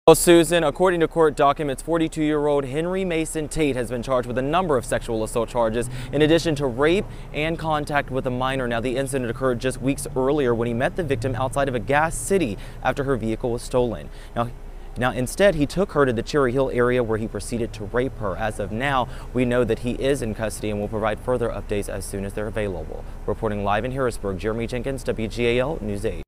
ARREST: Man accused of raping 17-year-old girl
Police say they have made an arrest in the rape of a 17-year-old girl in Harrisburg. Watch WGAL's Jeremy Jenkins' report above.